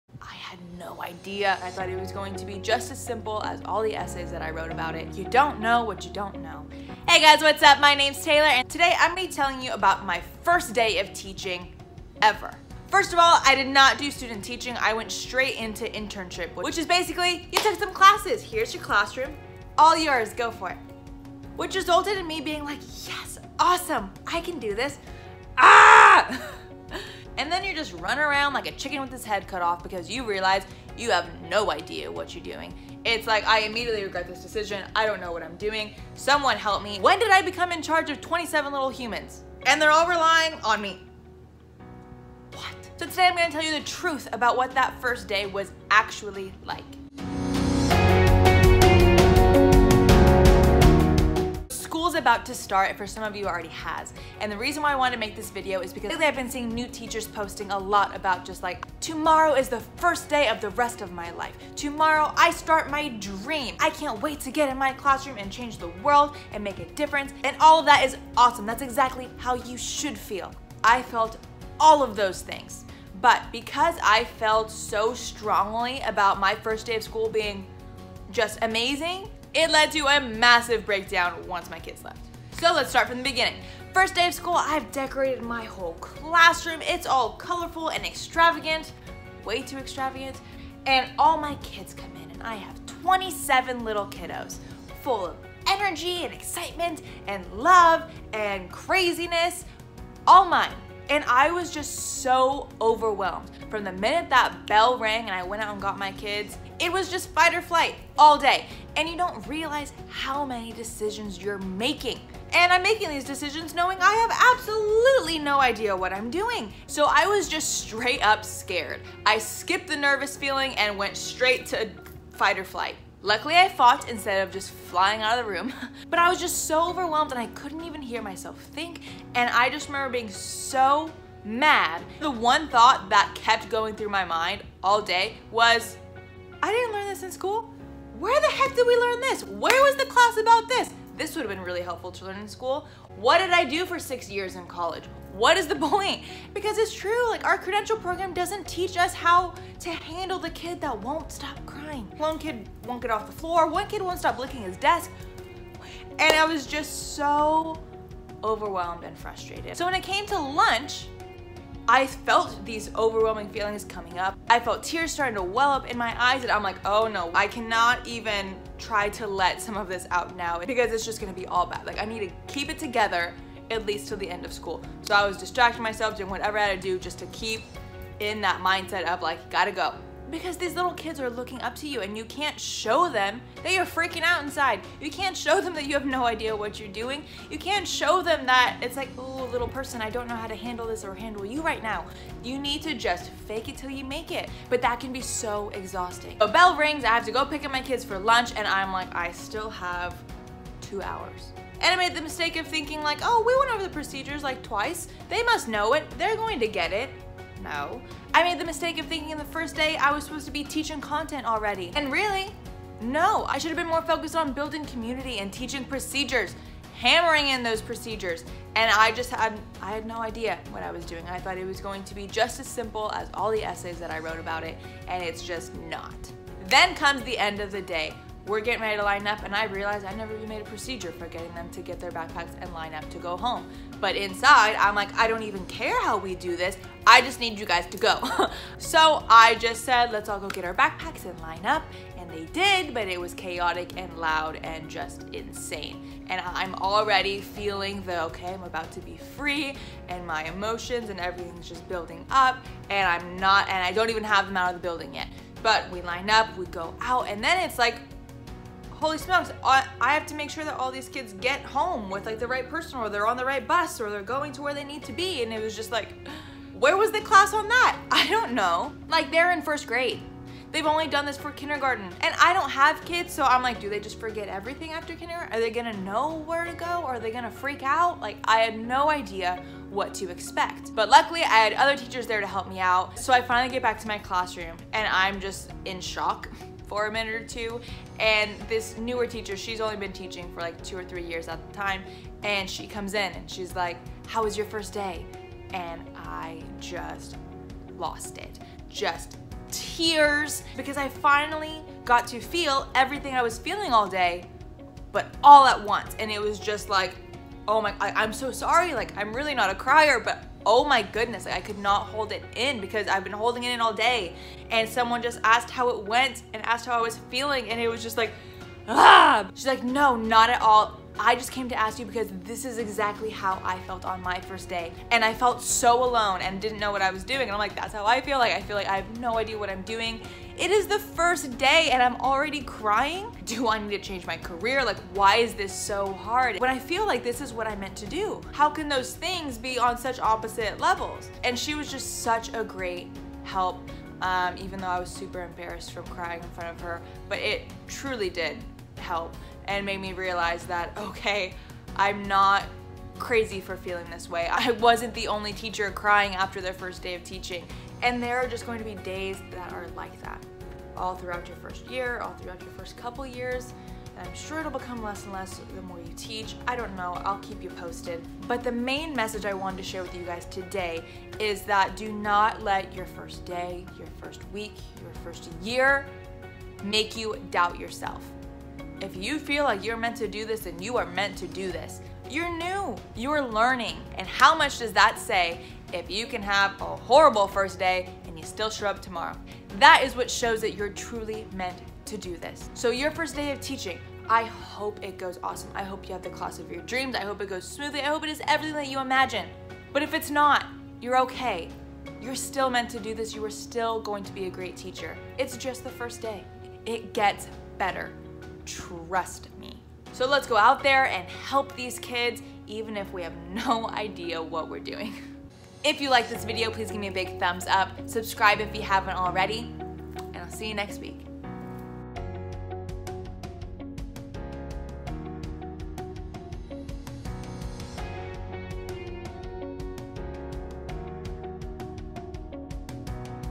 FIRST YEAR TEACHER // THE TRUTH About My First Day Of Teaching!
Hey y'all, the school year is about to begin and for some of you it already has so today I thought I would share my experience from my first day of teacher, ever!

Spoiler Alert, it was rough! But I wanted to share this, not to complain, but in hopes to relate to some of you! Teaching can be brutal at times and its easy to feel alone like I did when I was just starting out. So this is to show you that even if you have a rough start, it'll get better!

Looking forward to sharing this full upcoming year with y'all!